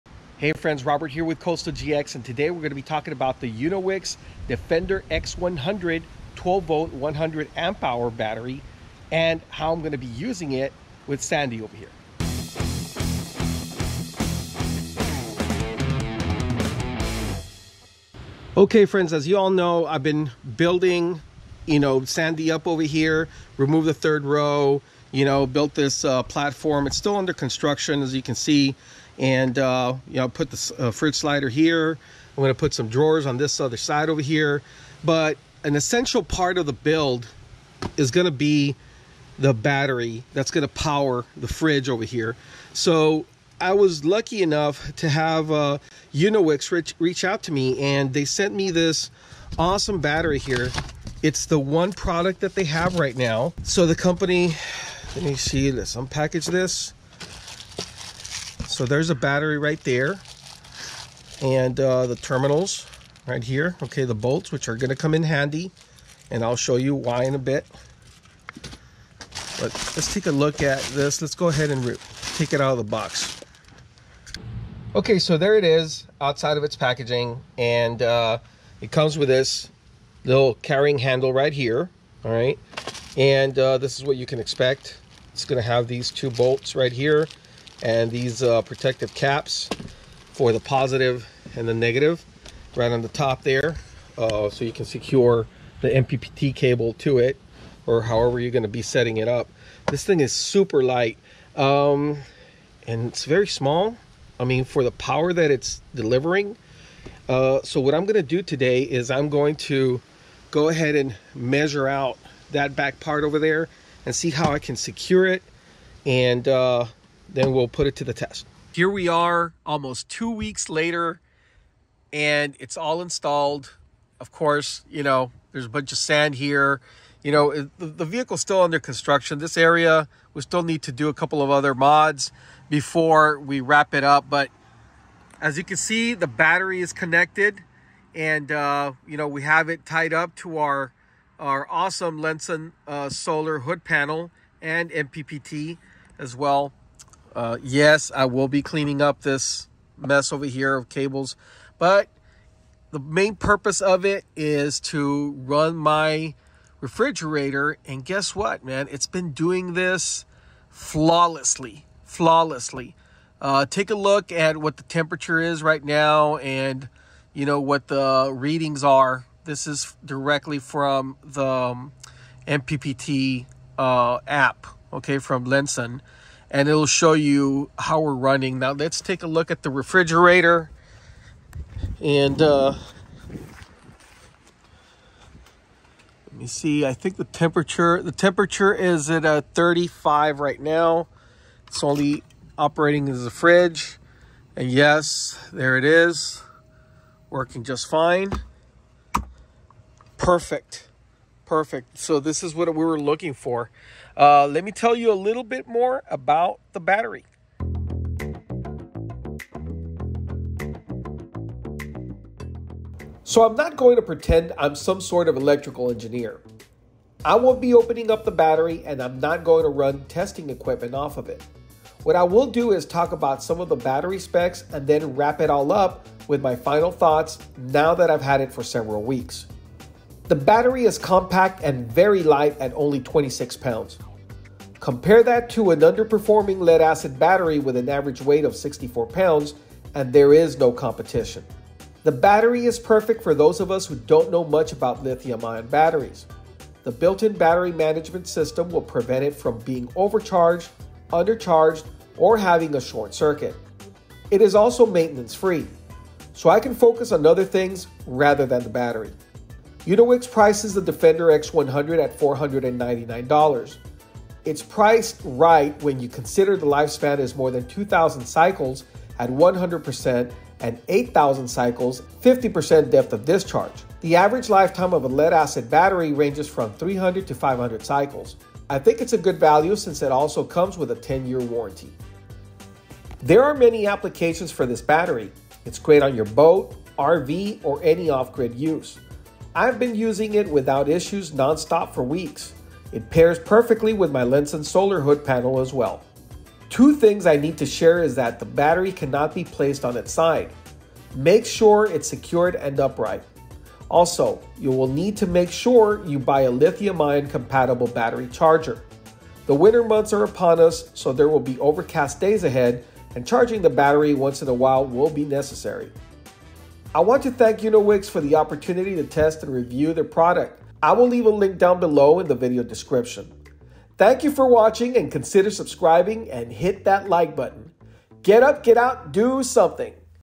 Overlanding with a Lithium Battery Setup: Unowix LiFePO4 12v 100AH
Welcome to my Overlanding Lithium Battery Setup review of the Unowix LiFePO4 12v 100AH battery. In this video, I'll discuss the specifications of this battery, as well as provide a full unboxing and review.

If you're looking for a lithium battery for your next overland adventure, then the Unowix LiFePO4 12v 100AH is the perfect battery for you! This battery is durable, has a long lifespan, and is perfect for powering your gear in the backcountry. I love this battery and have used it on several overland trips, and I can highly recommend it for anyone looking for a battery for their next overland trip!